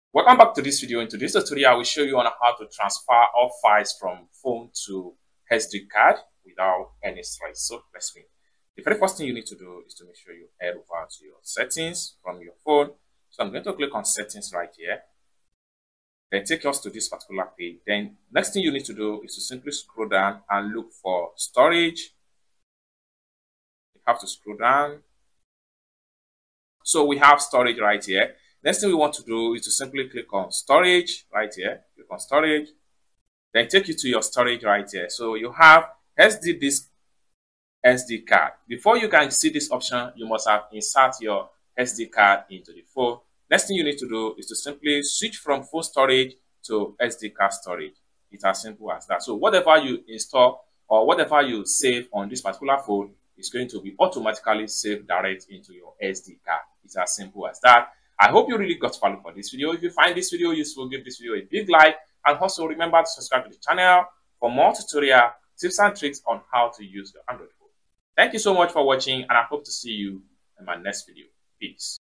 How to TRANSFER all Files From Phone to SD CARD!!
In this video, we'll show you **how to transfer all your files from your phone to an SD card** effortlessly. Whether you're looking to free up space on your device or simply want to back up your important data, this step-by-step guide will help you get the job done quickly and easily.